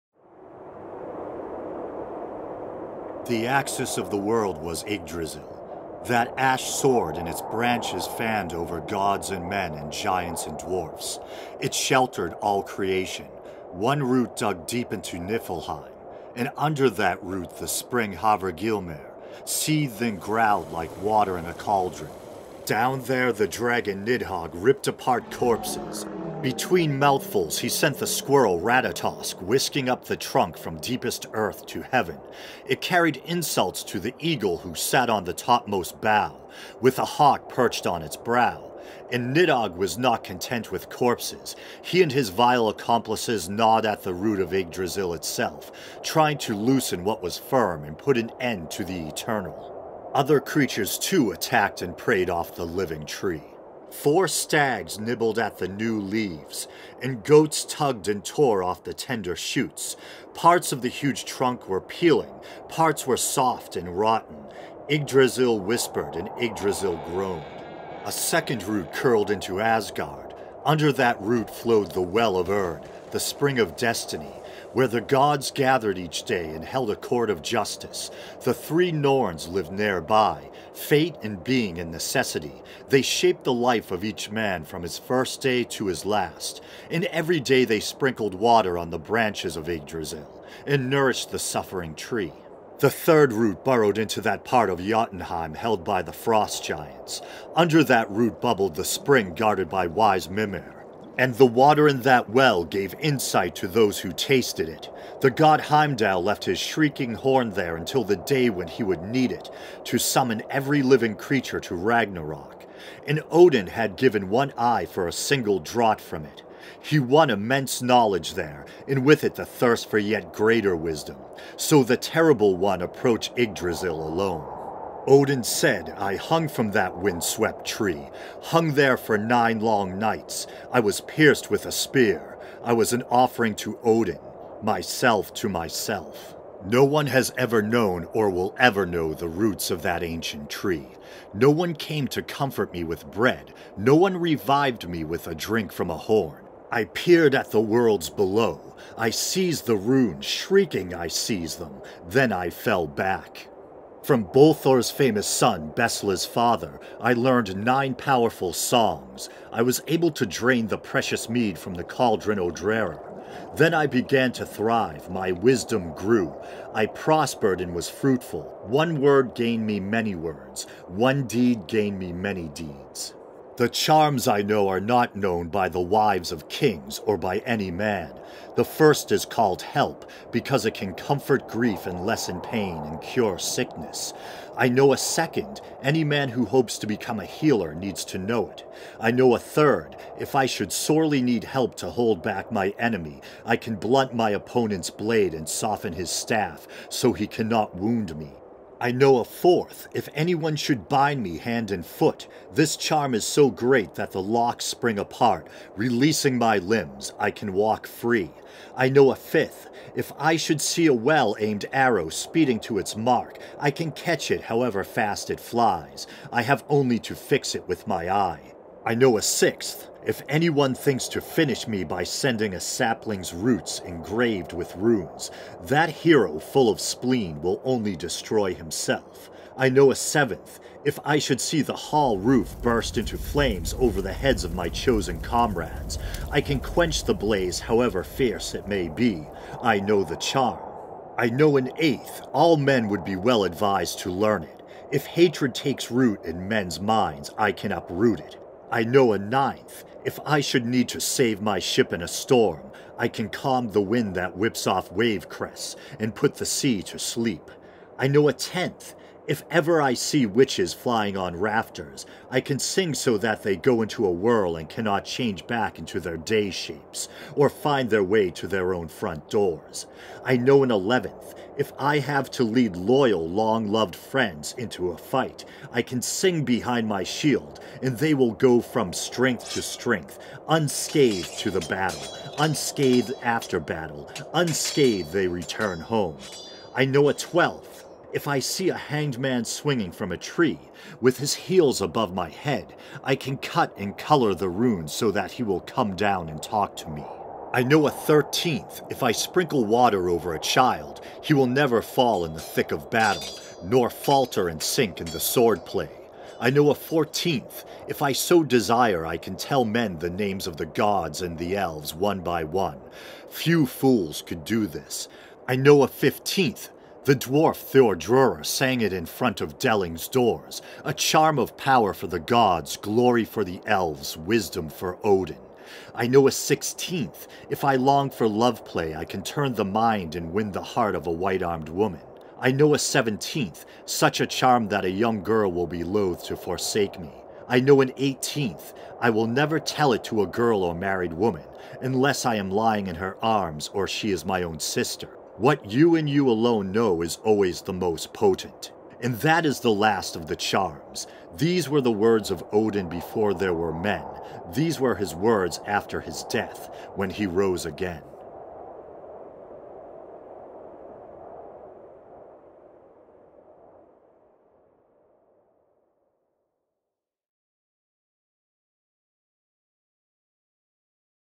The Lord of the Gallows (A Reading from The Norse Myths)
A reading from Kevin Crossley-Holland's The Norse Myths.